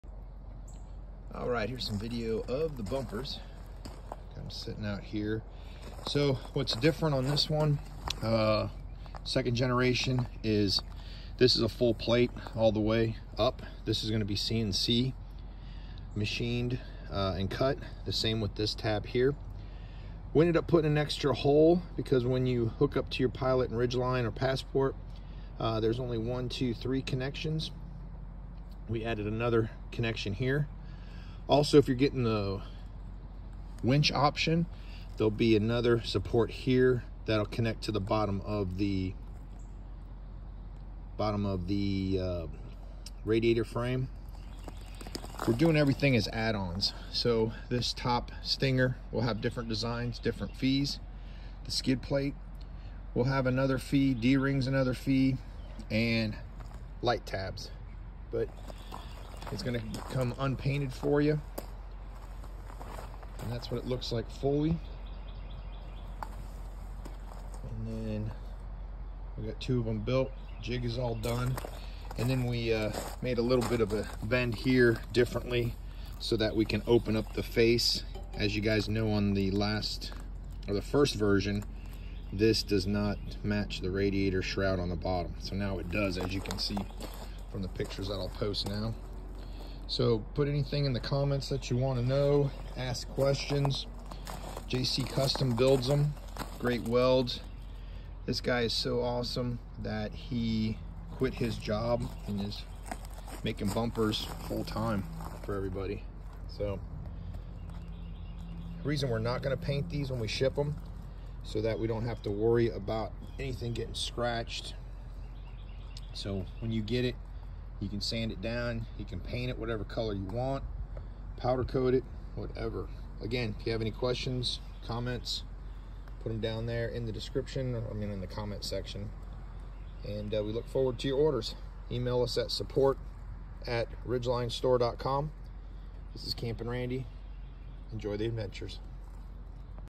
Pre-Runner Off-road Bumper Honda Ridgeline Passport The Ridge Store WIY with WINCH
We are now The CampingRandy™ Store. We have been working with Honda in compliance with our company name. Effective April 14th, 2023 we are now The CampingRandy™ Store.

Honda Ridgeline PreRunner Bumper V2

NEW DESIGN with Optional Upgrade Features. Will fit 2016+ Ridgeline, Passport, and Pilot. Ships LTL and weights 63lbs. Up to a Two to Six week build time before ships. You are also responsible for painting the bumper and trimming your Honda to fit the bumper. It will bolt direct to factory mounting holes. OFF-ROAD USE ONLY. *Bumper in pictures are the updated version with upgraded options. The video below is Ver1 Beta (not for sale)